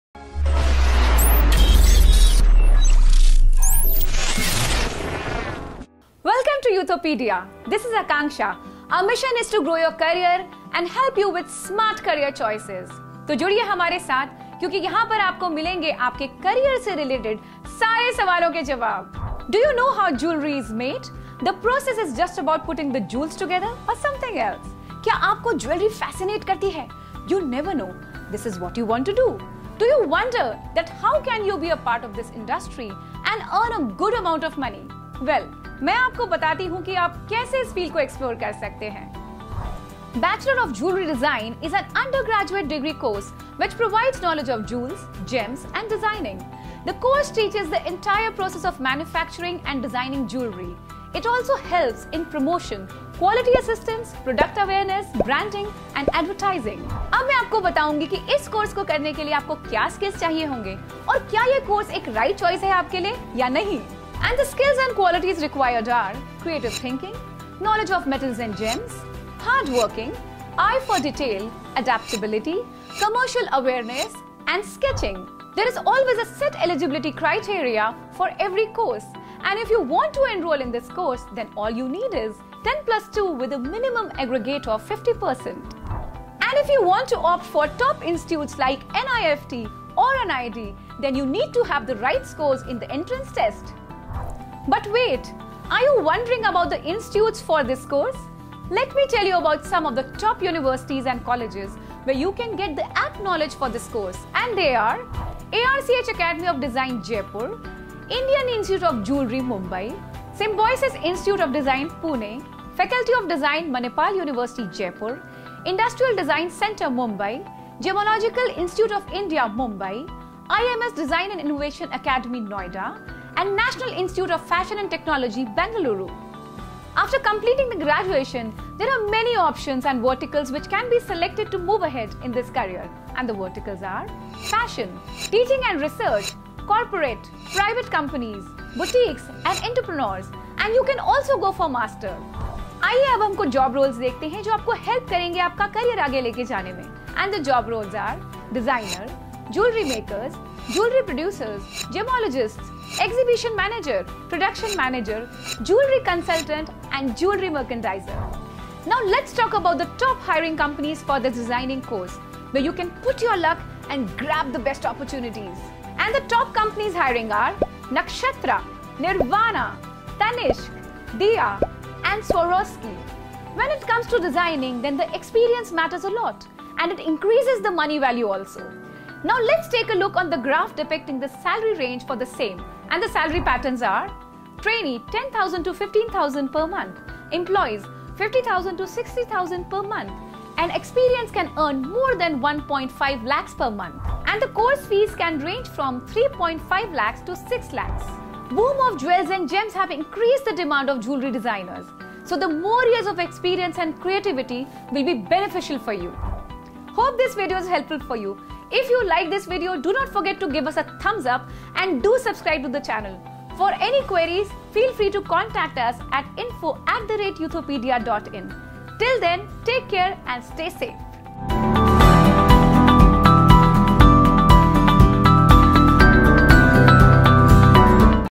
Career in Bachelor of Jewellery Designing
Get more information on Jewellery Design Courses, eligibility, scope, career options after a Bachelor of Jewellery Design, top placement companies, and salary.

Top Institutes for Bachelor of Jewellery Designing:
1. Jewellery Design And Technology Institute, Noida
2. Design Academy Of India, Delhi
3. Indian Institute Of Gems & Jewellery, Delhi
4. Design Academy Of India, Delhi
5. Indian Institute Of Gems & Jewellery, Delhi

Ask any question in the comment area and get it answered.